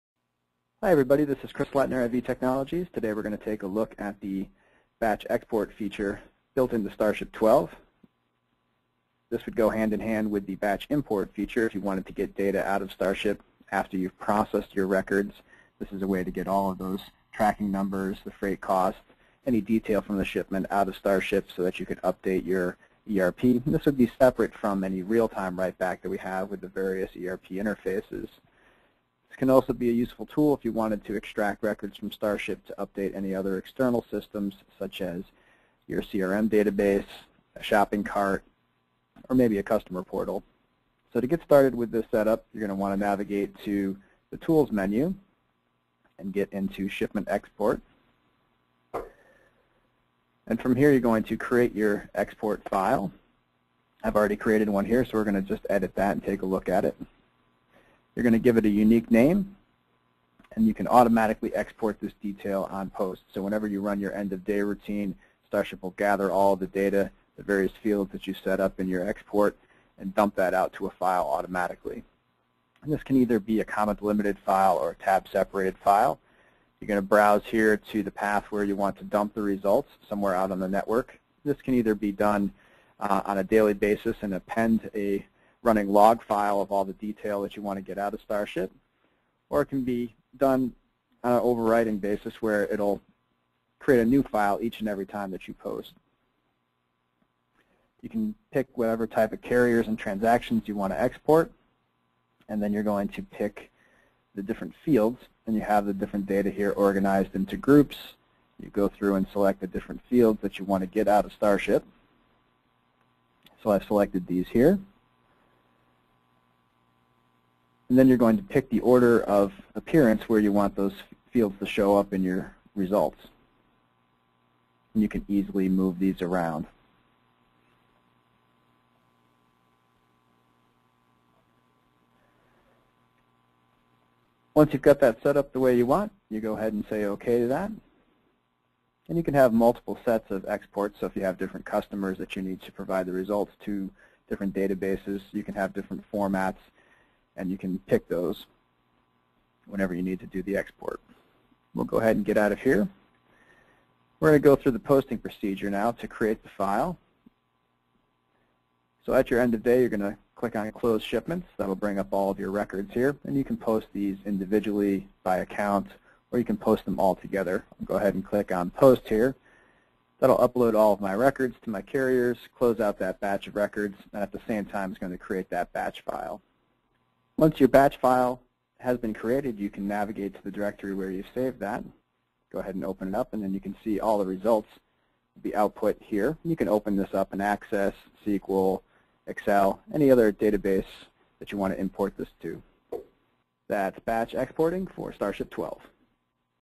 Shipping Software: How to export shipping related data in a batch
This batch export feature is in addition to real-time updates to Sage 100 ERP, Sage 500 ERP, Microsoft Dynamics GP, Epicor, Infor, Syspro and Macola.